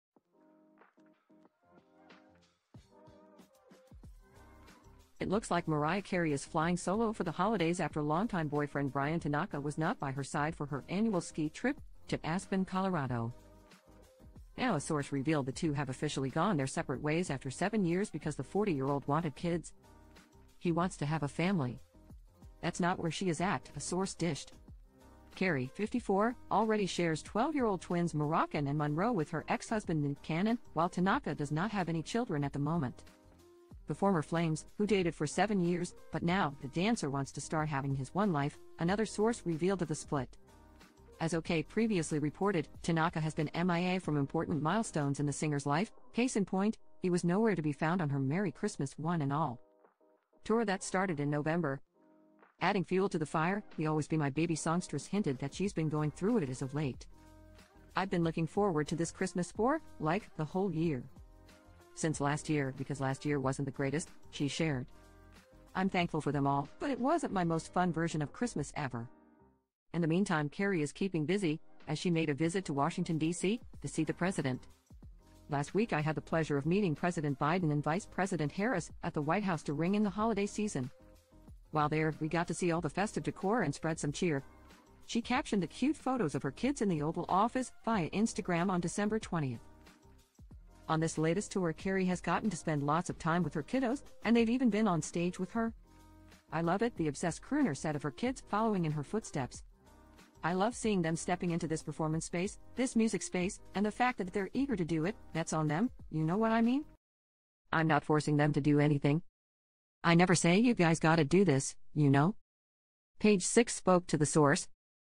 Mariah Carey and Bryan Tanaka's Cause of Split Revealed After Singer Goes on Annual Skiing Trip Solo
It looks like Mariah Carey is flying solo for the holidays after longtime boyfriend Bryan Tanaka was not by her side for her annual ski trip to Aspen, Colo. Now, a source revealed the two have officially gone their separate ways after seven years because the 40-year-old wanted kids.